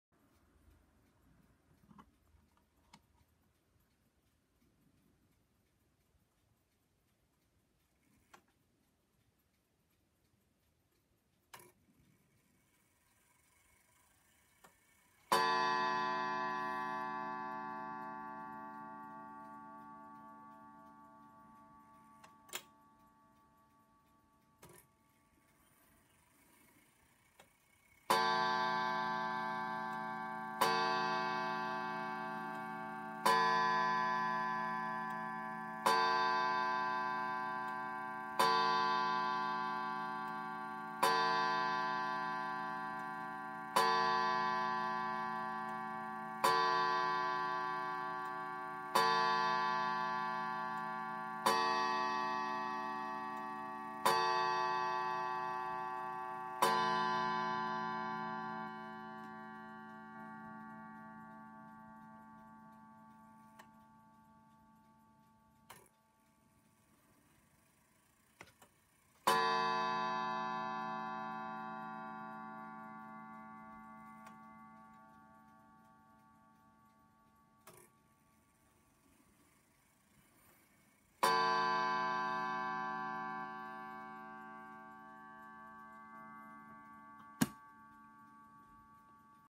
Smiths Enfield Bakelite ‘York’ striking clock, fully service and re-bushed, with original key
Video showing the hourly and half hourly strike, plus opening and closing of the bezel